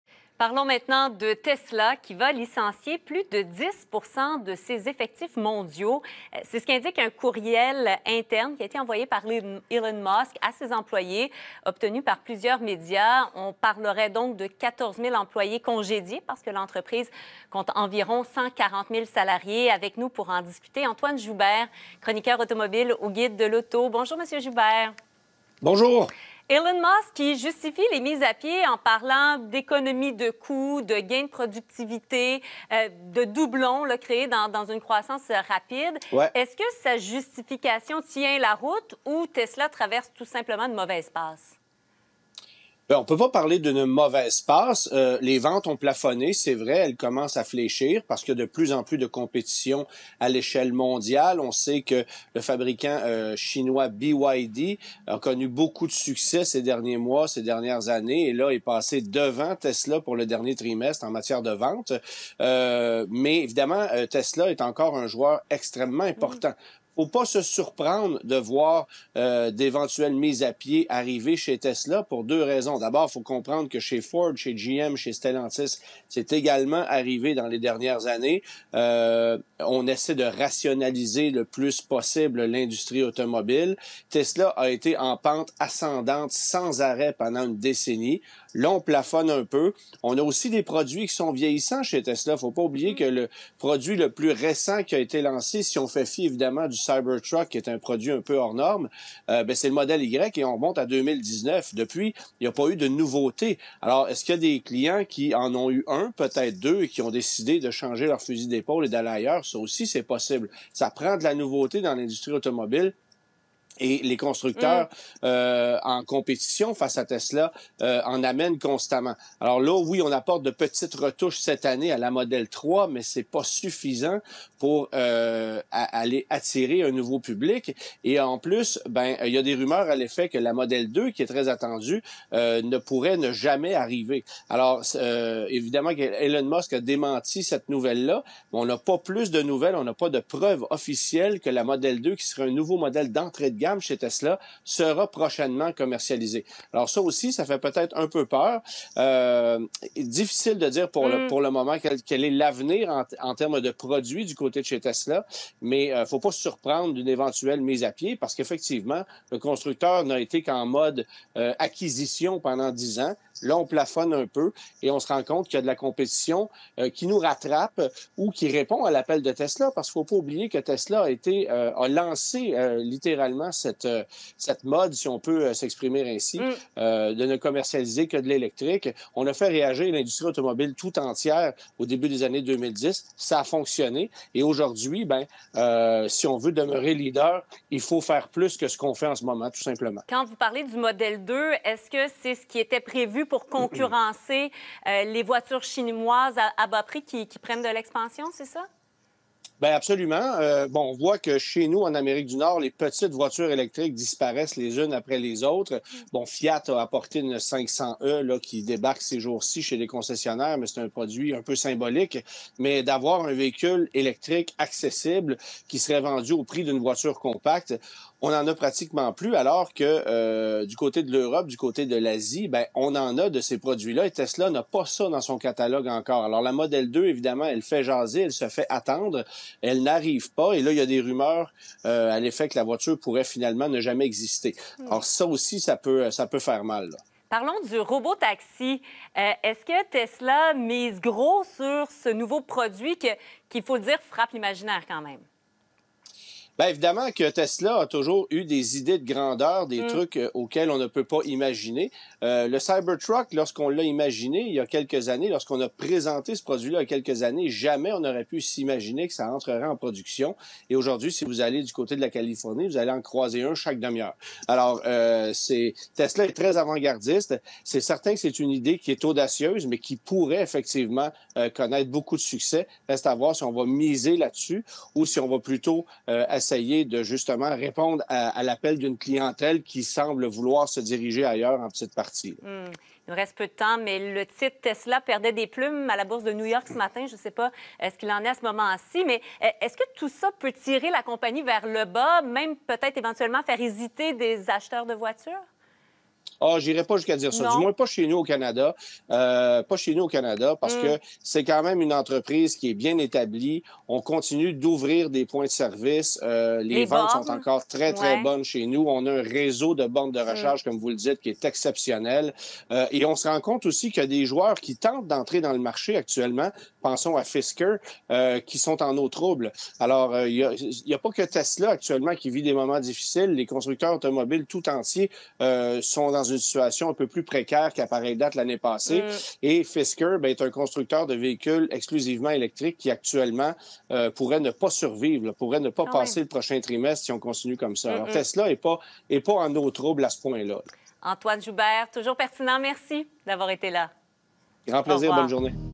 Tesla: réduction de 10% des effectifs mondiaux
DANS LES MÉDIAS | Tesla: réduction de 10% des effectifs mondiaux.  Antoine Joubert nous dit pourquoi.